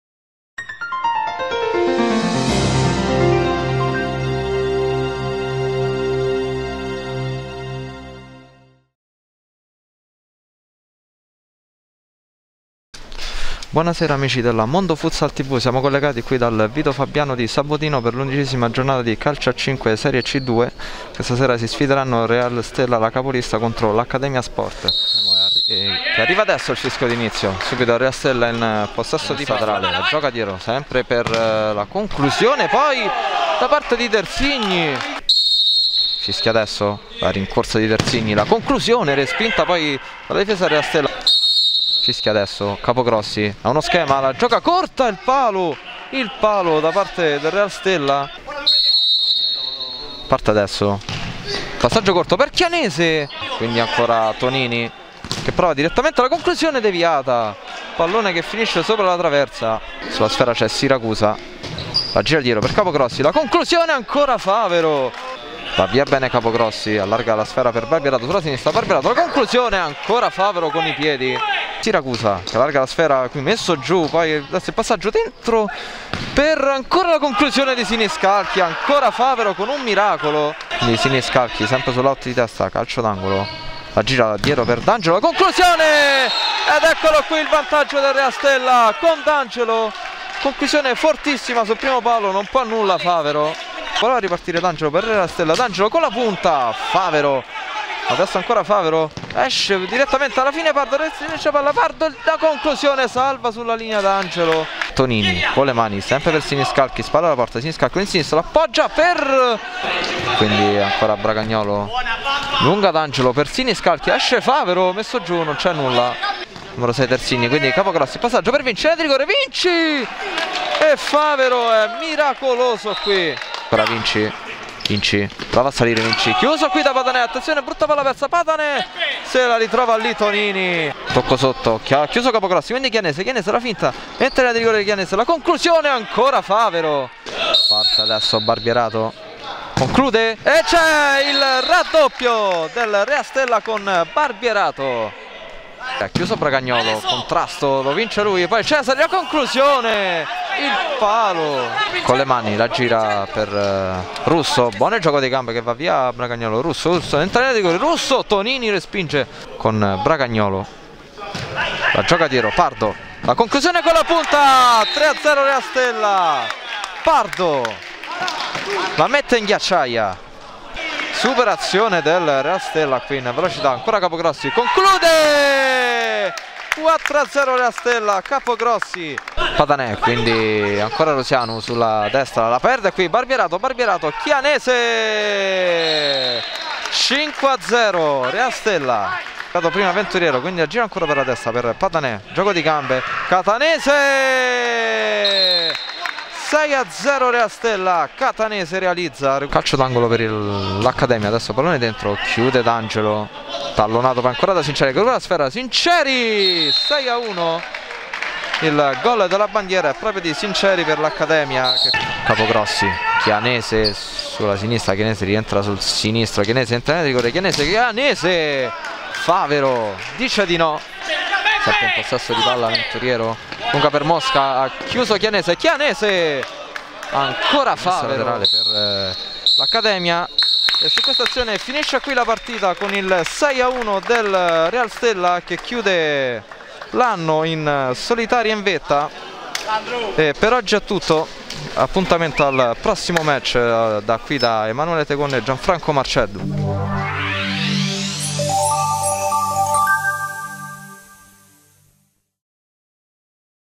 Serie C2: Real Stella vs Accademia Sport - highlights
Serie C2 gir.A, 11a giornata: vittoria per 6-1 quella dello Stella prima della sosta natalizia